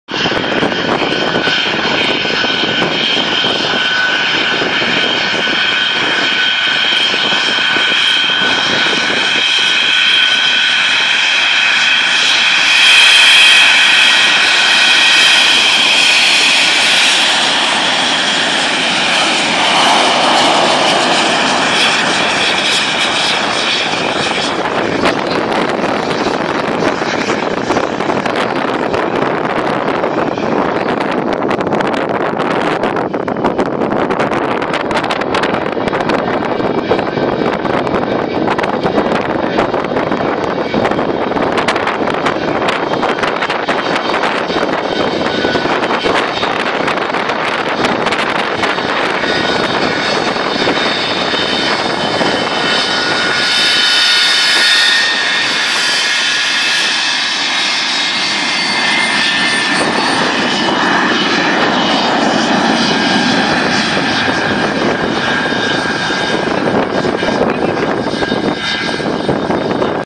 XS458 streatching her legs
running out for a fast run and taxi back